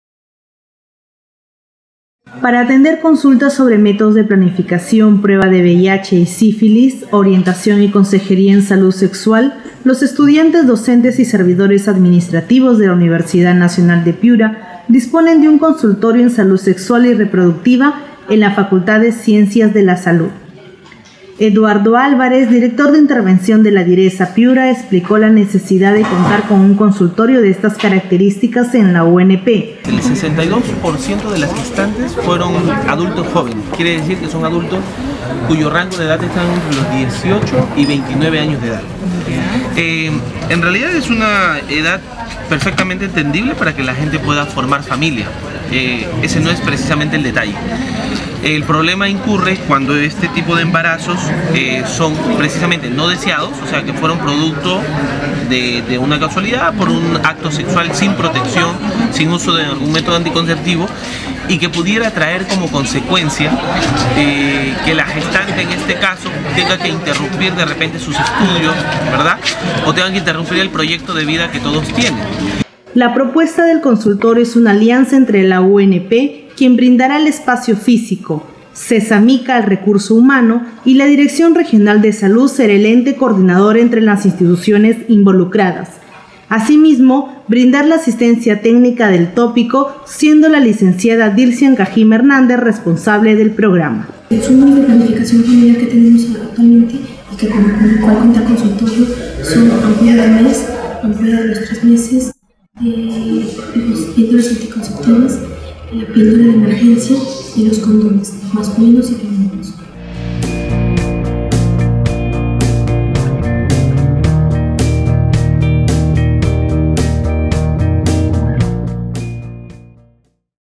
UNP APERTURA CONSULTORIO SOBRE SALUD REPRODUCTIVA
Para atender consultas sobre métodos de planificación, prueba de VIH y sífilis, orientación y consejería en salud sexual los estudiantes, docentes y servidores administrativos de la Universidad Nacional de Piura disponen de un consultorio en salud sexual y reproductiva en la facultad de Ciencias de la Salud.